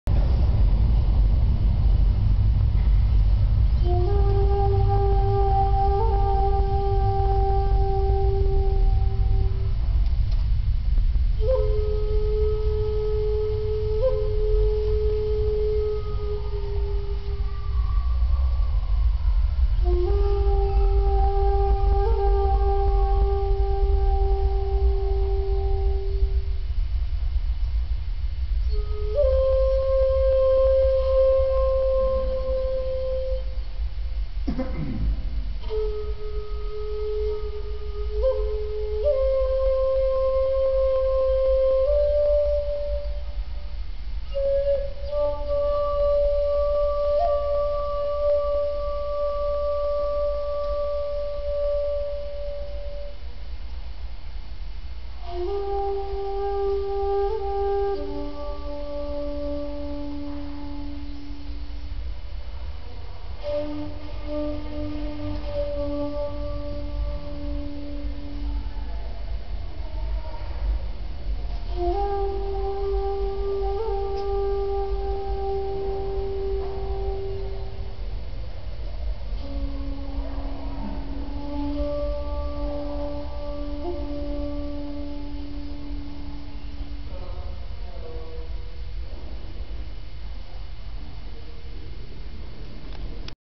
Sigwada Shakuhachi at Subway
playing my self made flute